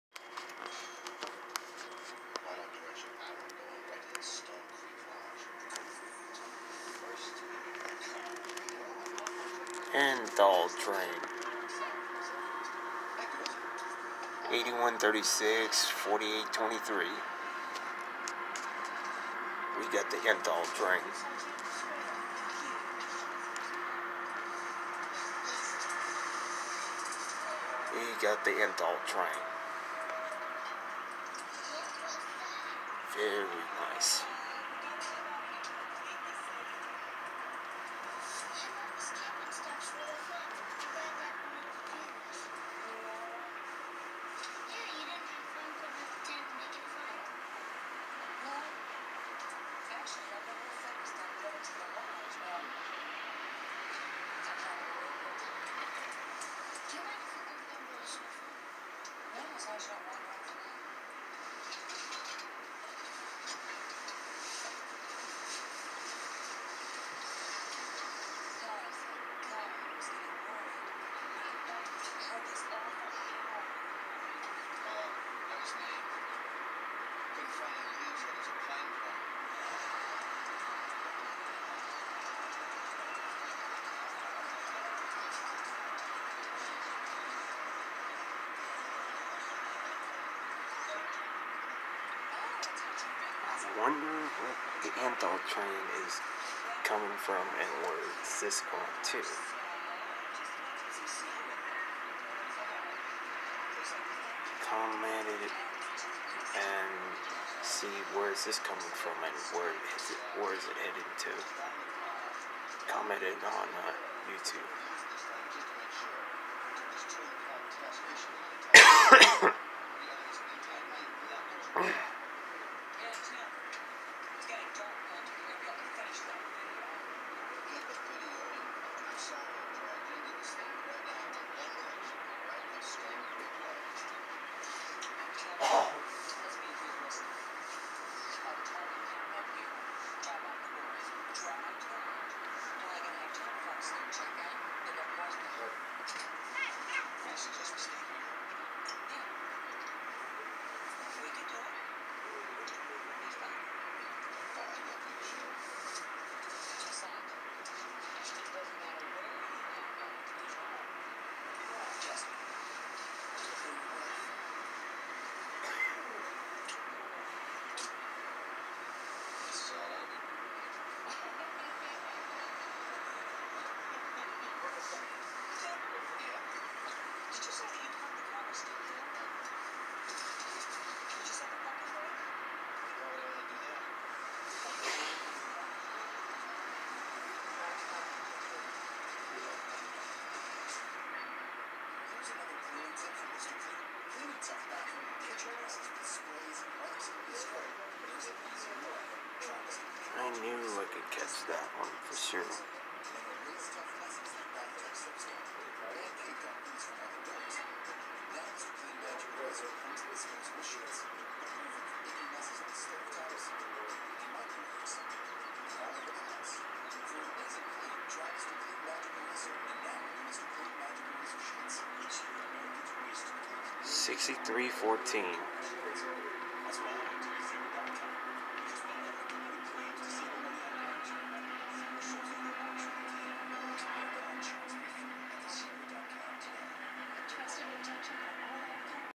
Union Pacific 8136 West @ West Sacramento 6/9/2019
UP #8136 (SD9043MAC) brings up the ethanol train heading westbound on the UP Martinez subdivision

Engine
UP 8136 EMD (SD9043MAC)
UP 4823 EMD (SD70M)
UP 6314 GE (AC44CW) DPU